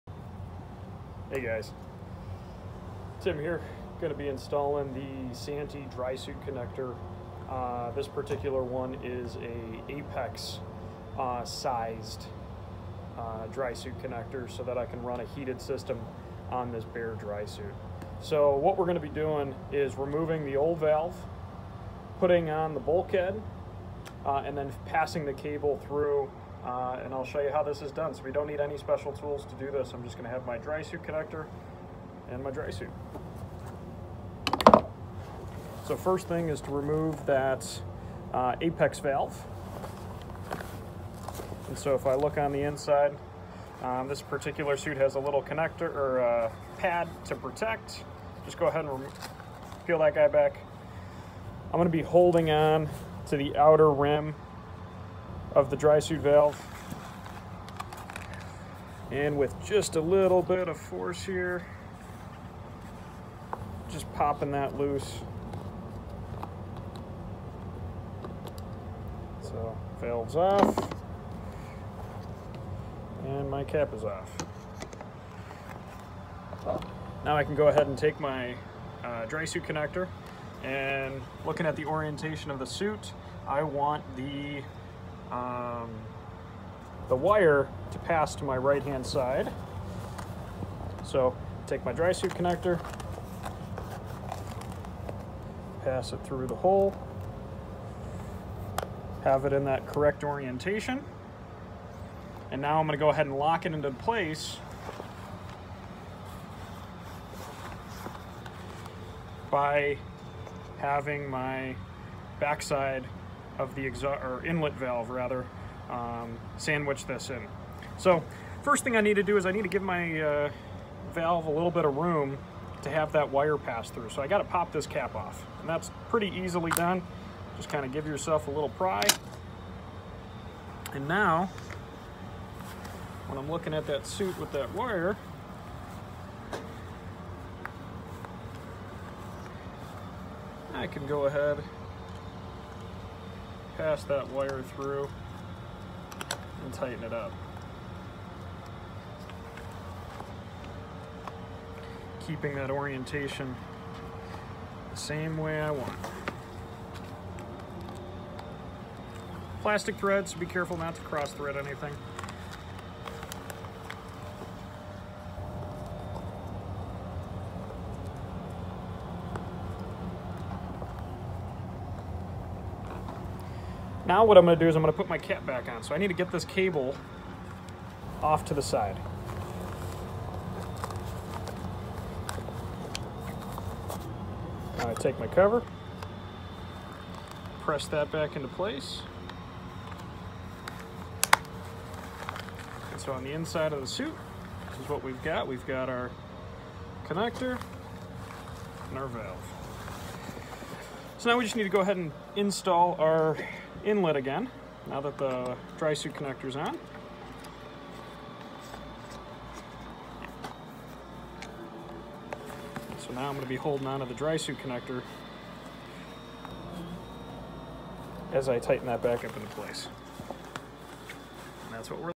Installing Santi Drysuit Connector on a Drysuit
A quick video on how to install the santi drysuit connector on a drysuit. This drysuit connector allows you to pass a heating wire from the external battery to the inside of the suit.